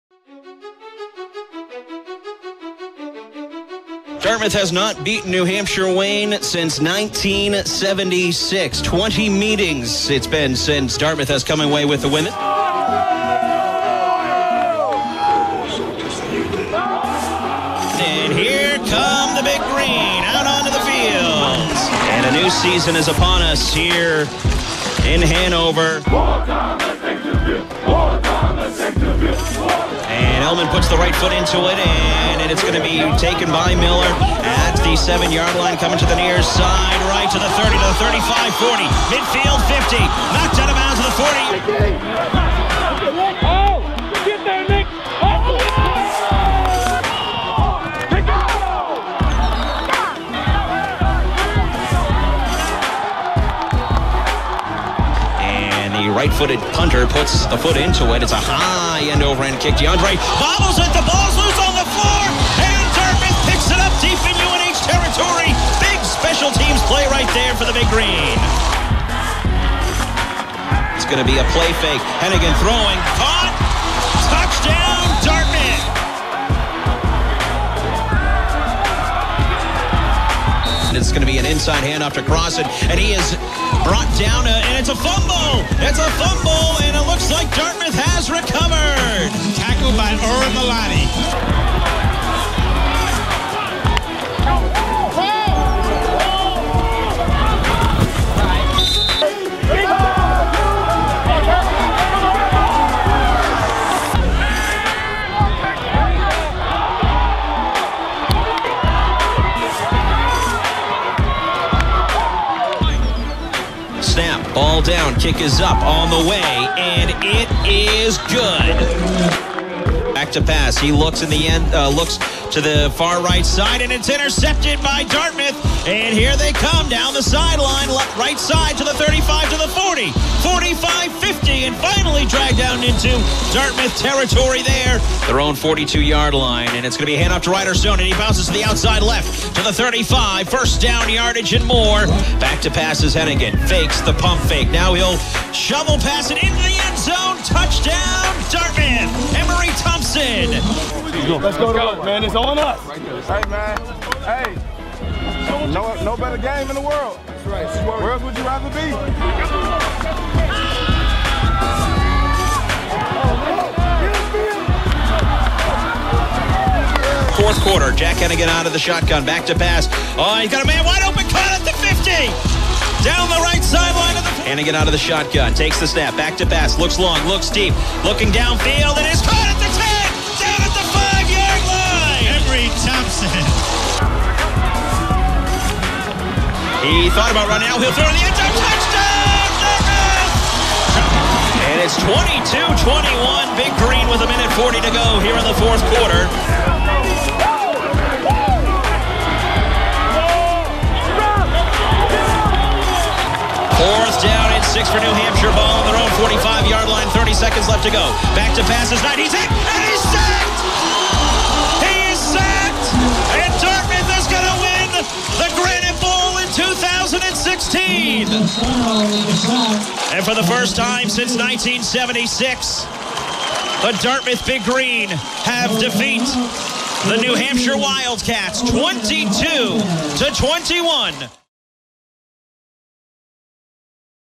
UNH Recap 2016 || Dartmouth Football
Take a look at Dartmouth's gutsy 4th quarter comeback over UNH!